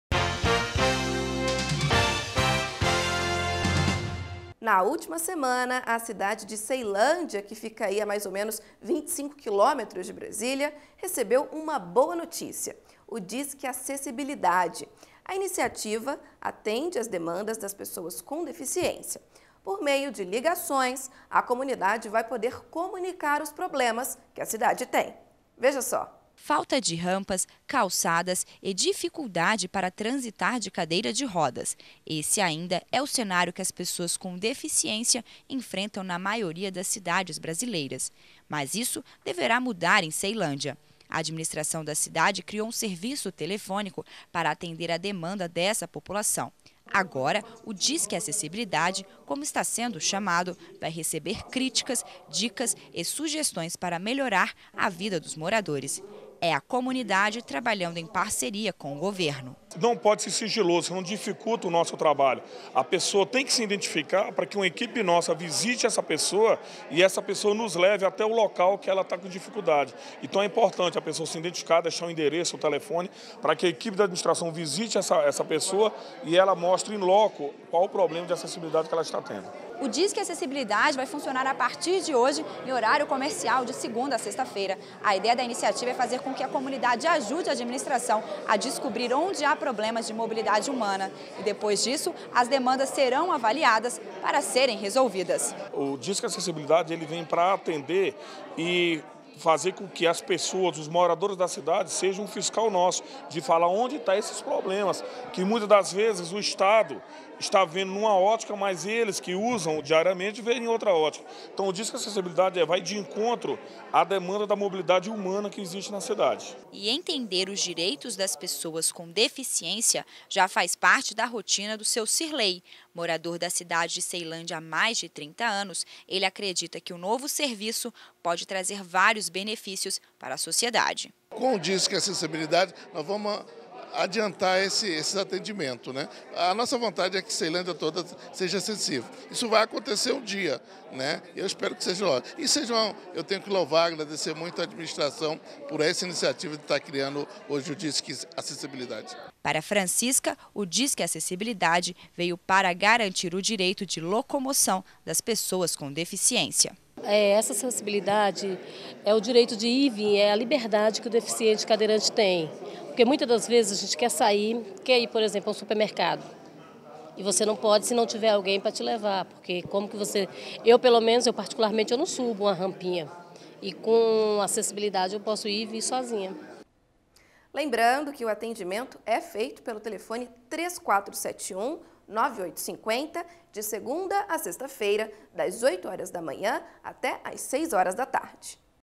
Ceilândia recebe "Disque Acessibilidade" para pessoas com deficiência
A cidade de Ceilândia, localizada a 25 quilômetros de Brasília, recebeu uma boa notícia: o Disque Acessibilidade. A iniciativa atende as demandas das pessoas com deficiência. Por meio de ligações, a comunidade vai poder comunicar os problemas que a cidade tem.